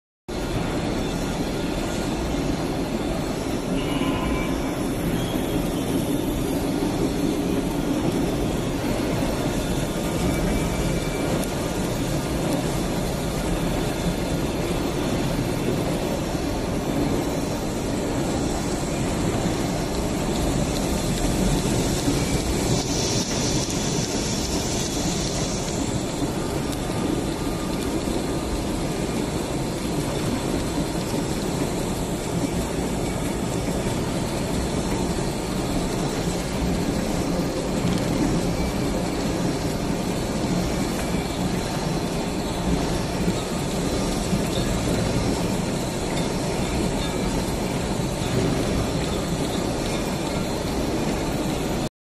Automatic Electric Plastic Film Paper Roll Slitting Cutting Rewinding Machine
This slitting machine can be used for slitting and rewinding PE, OPP film roll, paper roll and other material. This machine can slit material into very small size, such as 5mm or 10mm,   welcome for your inquiry.